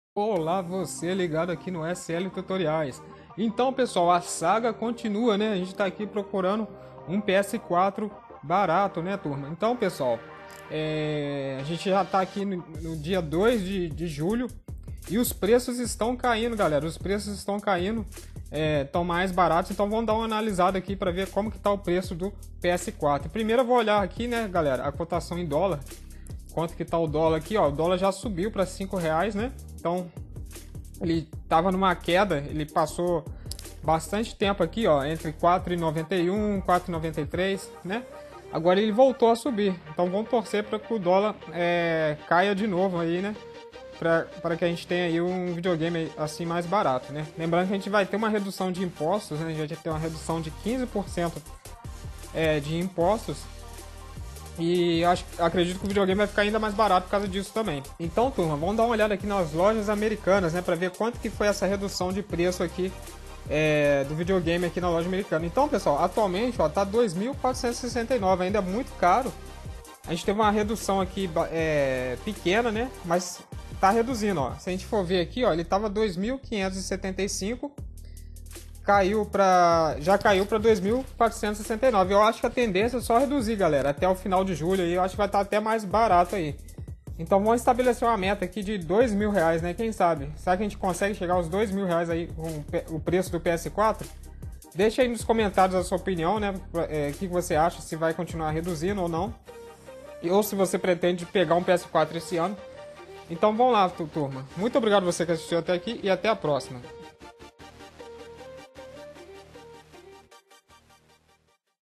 OBA!!! PS4 CAINDO DE PREÇO AGORA - PlayStation 4 SLIM 1TB BUNDLE (2021)
Fala pessoal! É real, o PS4 realmente está caindo de preço! Veja neste vídeo como estão os preços do PS4 no início de julho de 2021!
↓ Mais Informações ↓
★VAMOS: Curtir ✔Comentar ✔Compartilhar ✔Inscrever-Se ✔★